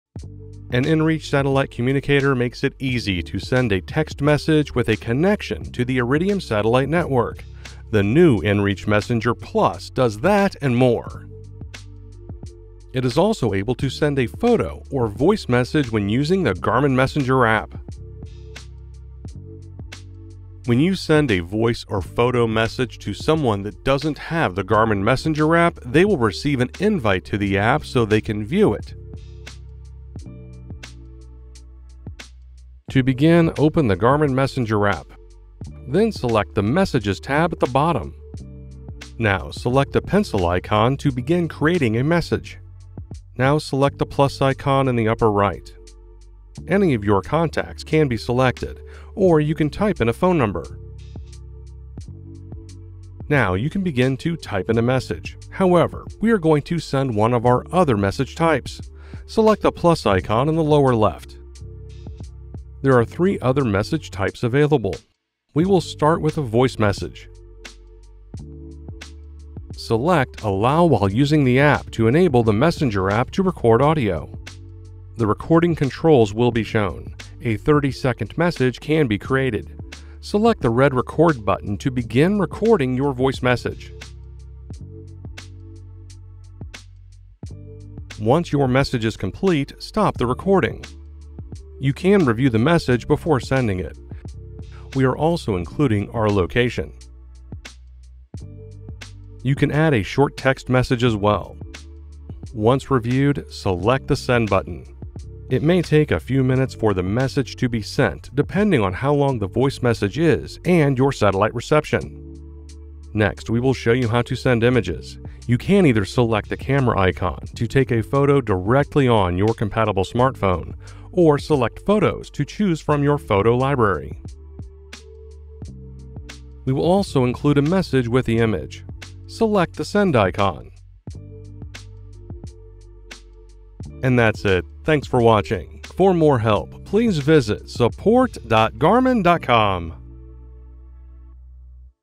inReach® Messenger Plus | Sending Messages | Garmin Support
Learn how to easily send a text, voice or photo message with the inReach® Messenger Plus satellite communicator, using the Garmin Messenger™ app on a compatible smartphone.